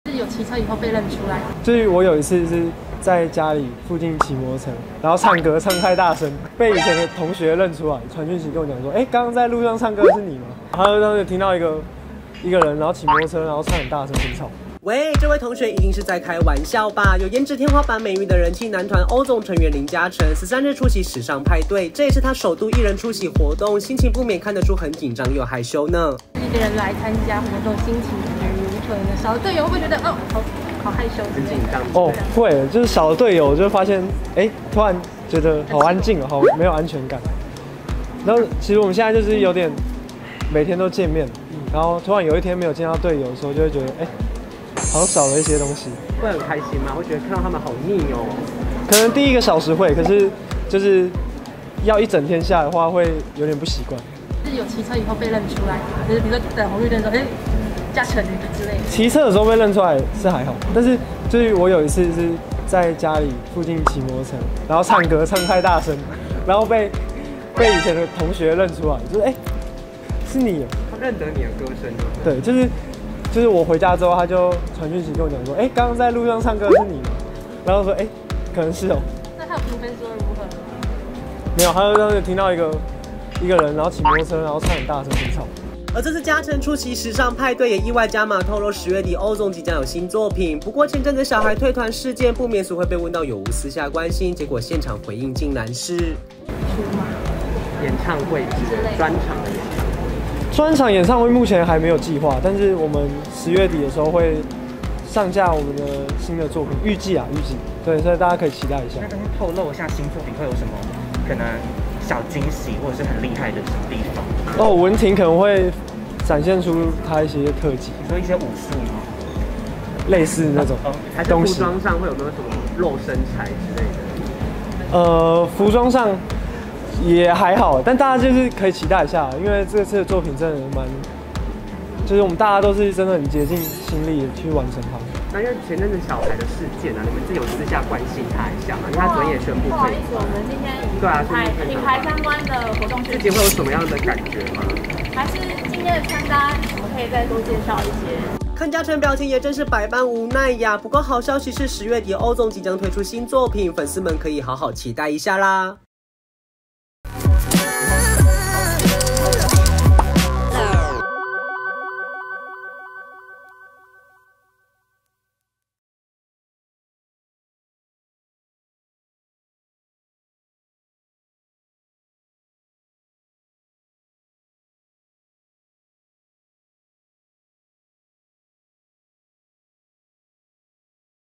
林佳辰驚喜曝Ozone將有新作 被問小孩事件竟然「這反應」
從《原子少年》出道的人氣男團Ozone成員林佳辰，13日出席時尚派對，首度一人出席活動難免看出一點緊張與害羞感，「其實我們現在每天都見面，突然有一天沒有見到隊友的時候，會覺得好像少了一些東西。」